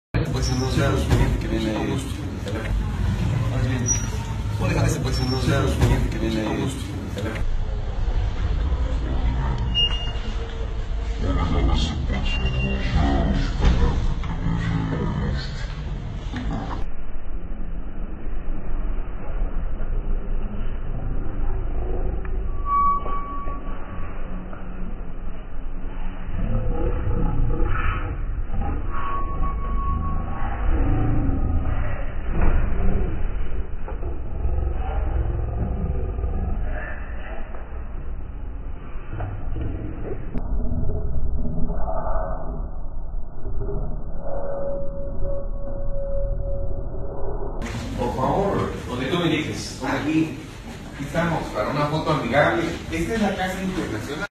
EPN saca un arma frente al presidente de Guatemala??
El presidente de México, Enrique Peña Nieto, en este video parece que saca lo que aparenta ser un arma. Ustedes que opinan?